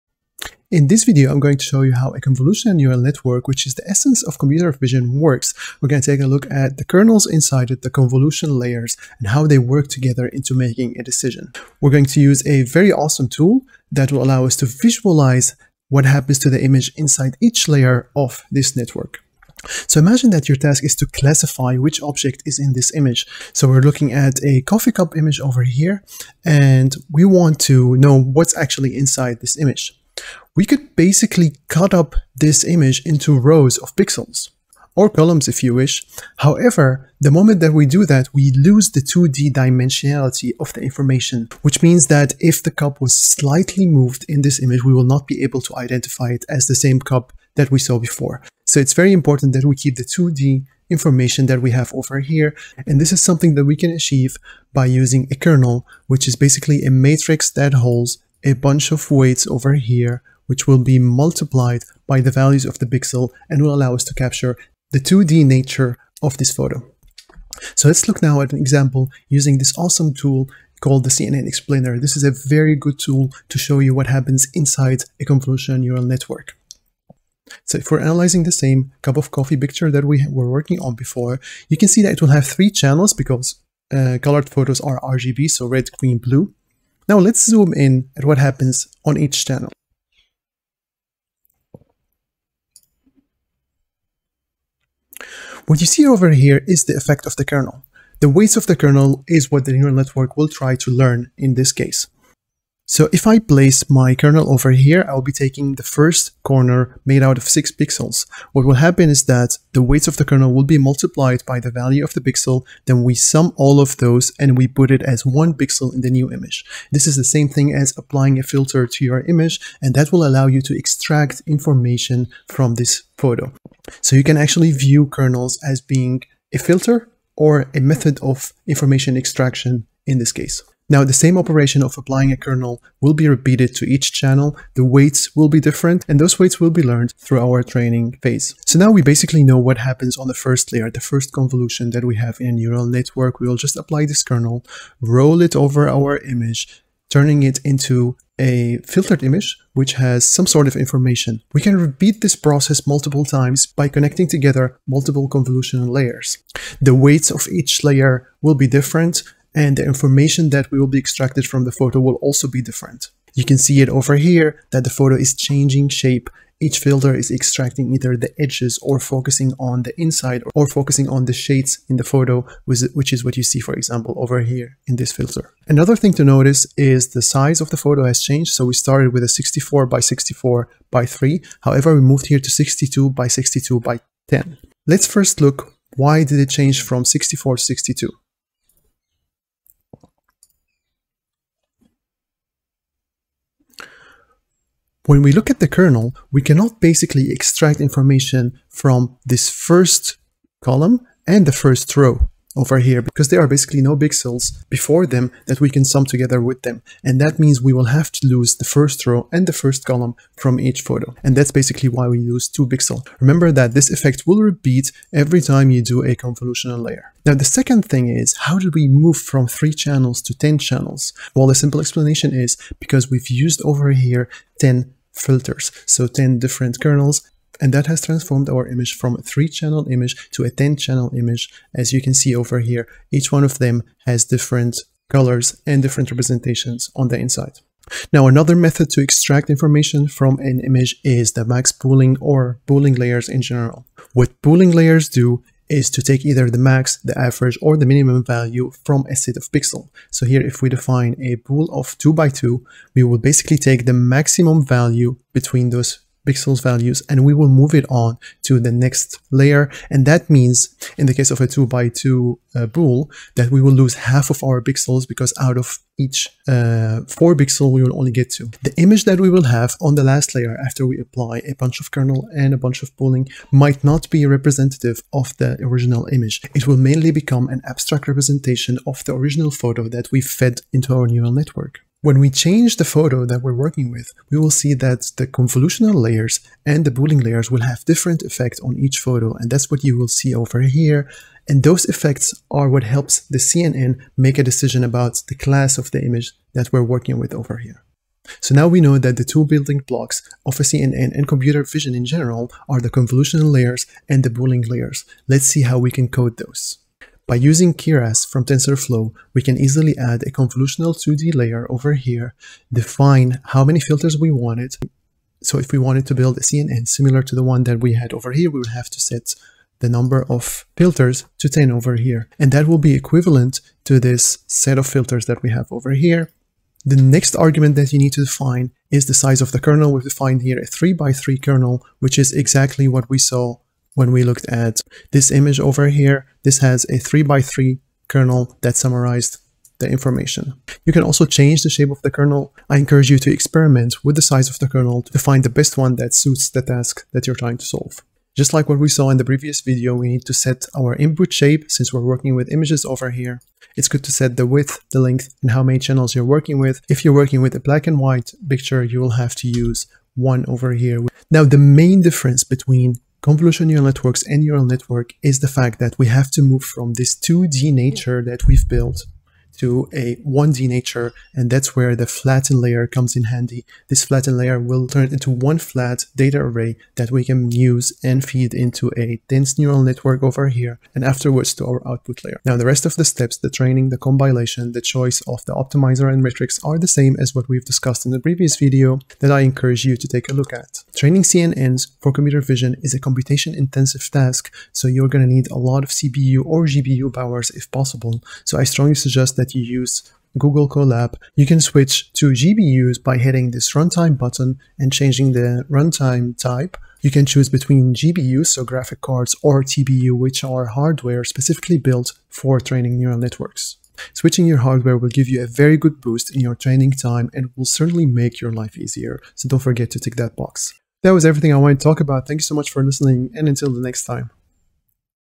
Python convolutional neural network code + cnn explainer
In this video I’m going to show you how a convolutional neural network analyzes an image. We are going to take a look at kernels, convolutional layers and how they work together to make a decision in a CNN. You can also see how to code a CNN for computer vision in the colab notebook.